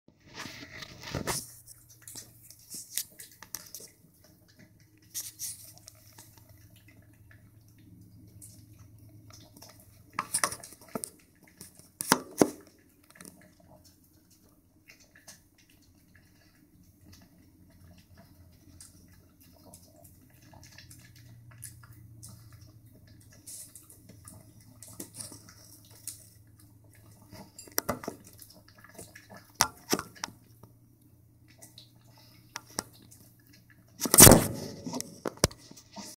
Boston terrier baby SKY meal time
Baby SKY eats her meal. So hungry.